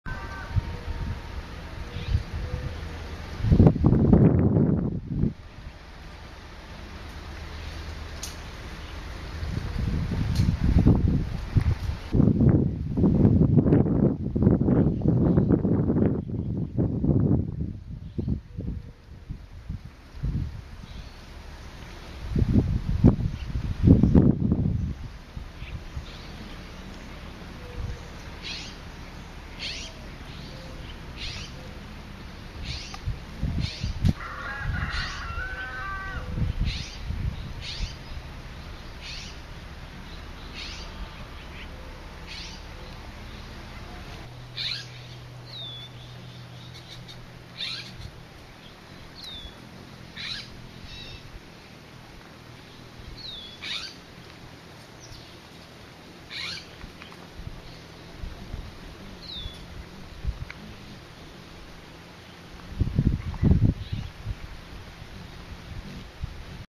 Hummingbird on pink Zauschneria
An Anna's Hummingbird working the flowers of a Pink Zauschneria, California Fuchsia,  over a period of a half of hour at Las Pilitas Nursery.